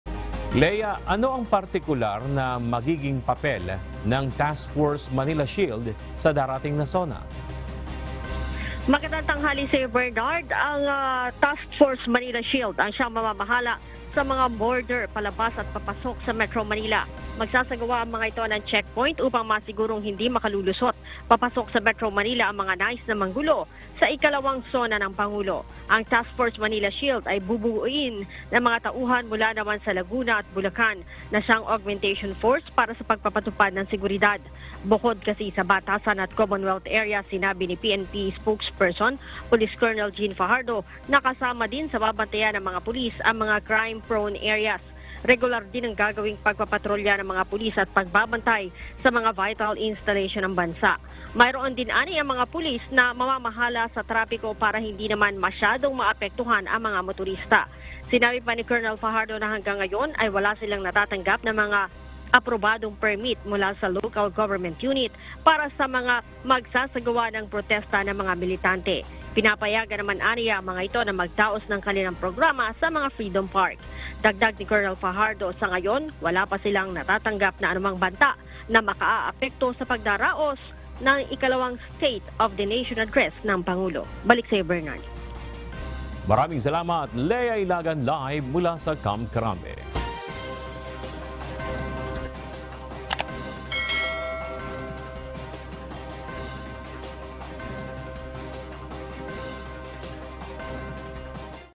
Task Force Manila Shield, bubuhayin ng PNP para sa seguridad sa NCR sa July 24
Bubuhayin ng Philippine National Police ang Task Force Manila Shield kaugnay ng idaraos na State of the Nation Address ni Pangulong Ferdinand Marcos Jr. sa July 24.

Ayon sa PNP, layunin nitong matiyak na hindi sila malulusutan ng mga taong nais manggulo sa nasabing okasyon.